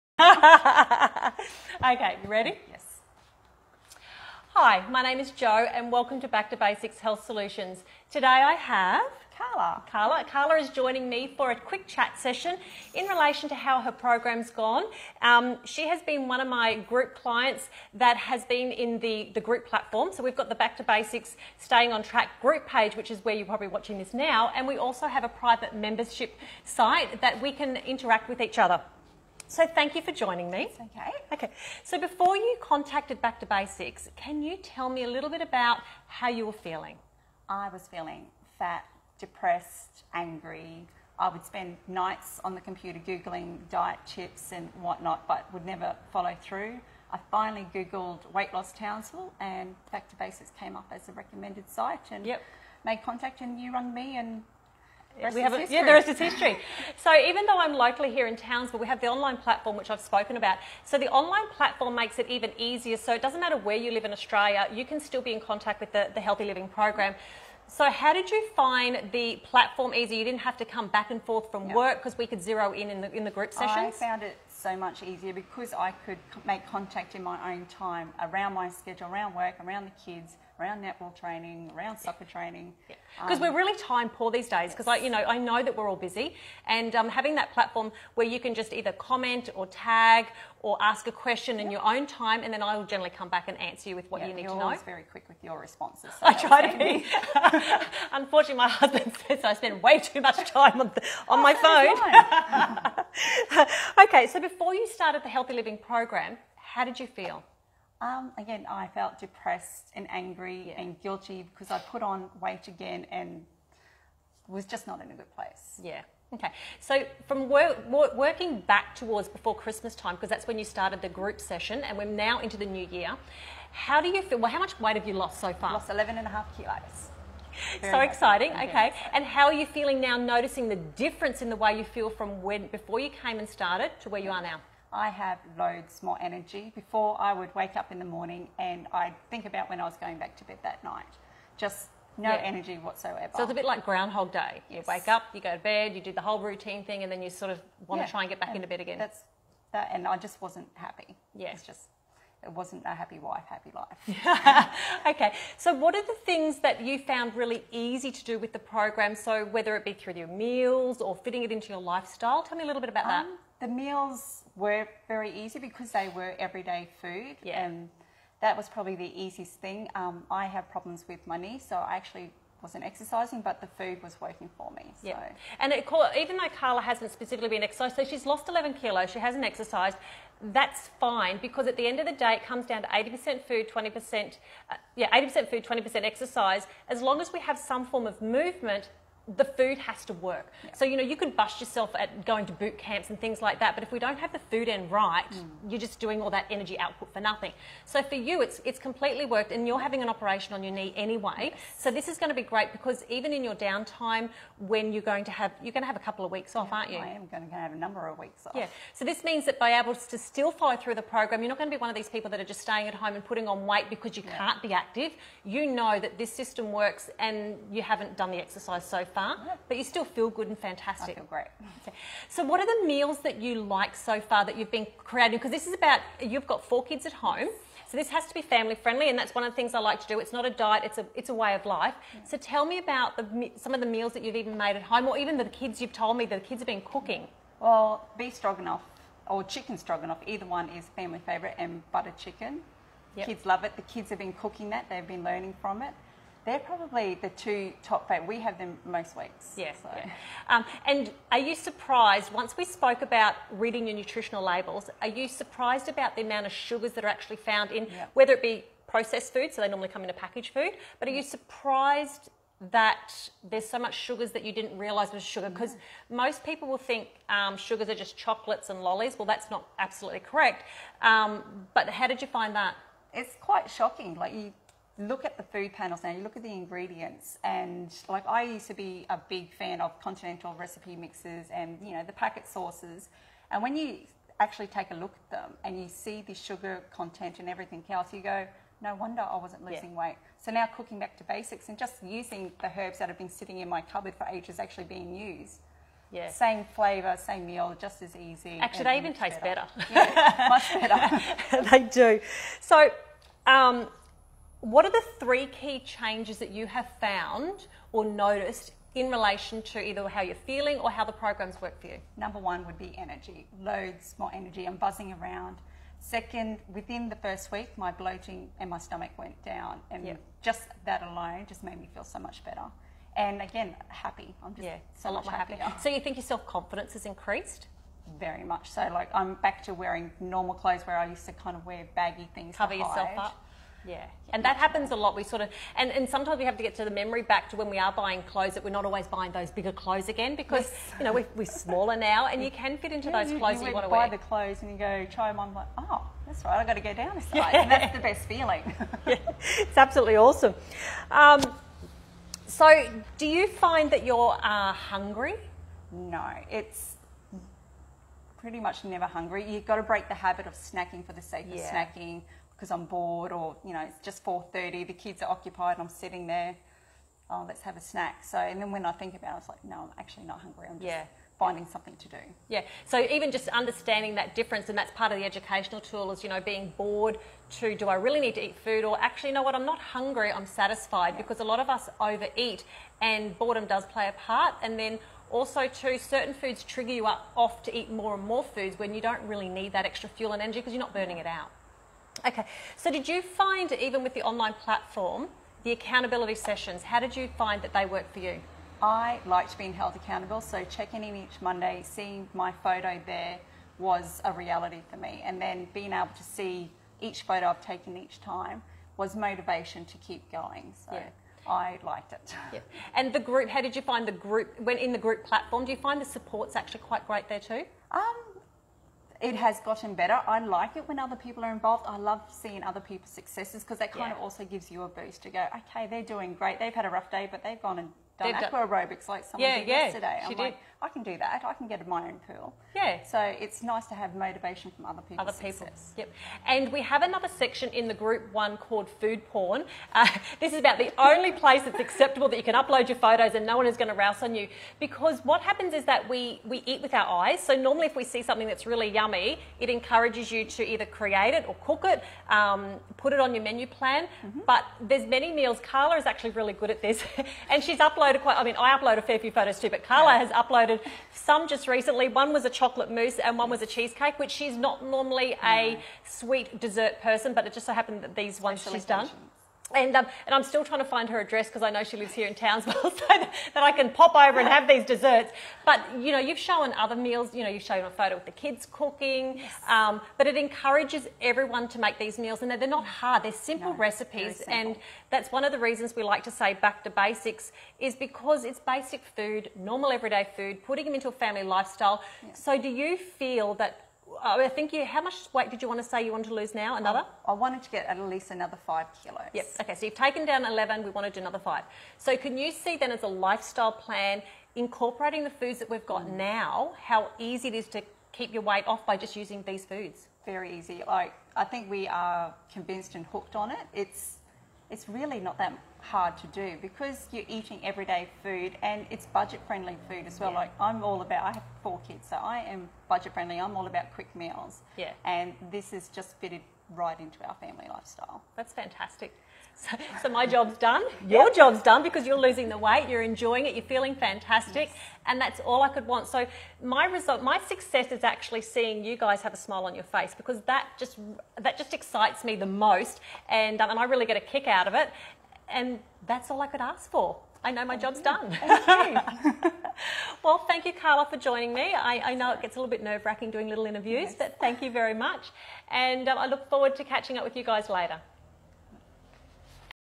Carla's Health Journey "I was sick of feeling fat and depressed... I was angry all of the time"
Before Carla started working with me she felt fat, depressed and angry that she had let her weight get out of control.

She was consistently tired all of the time and as soon as she would wake up in the morning she was counting the hours until she could go back to bed!

Her stomach was bloated and I remember her telling me it looked like she was always 4 months pregnant.

Lets fast forward Carla's health journey...

When I caught up for a chat with Carla during this video she had lost 11.5kg and still wanted to drop a couple of extra kilos...

Drum roll please... I am so excited for her as she has now reached 17.9kg!

And is now happy to maintain her weight.

For Carla it was not just about losing the weight, she desperately wanted more energy and needed help with making the right food choices and was totally surprised at how much sugar was in some of the foods she was eating.

With a family of six it was important to be budget friendly and to make meal time as easy as possible. Being a busy mum on the go with four growing children she wanted to create quick and easy meals everyone would enjoy eating.

Now armed with the knowledge of how to read nutritional labels, knowing what are the right foods to eat and creating a healthy eating plan for her and her family life has become much easier.

Even the kids have been cooking up a storm in the kitchen!

From when I first spoke to Carla her life has changed so much... she now has loads more energy, that bloated feeling in the stomach has long gone and her self confidence has had a big boost.

Helping people is the part I enjoy the most... hearing the positive impact on how people lives have changed for the better is like icing on the cake for me.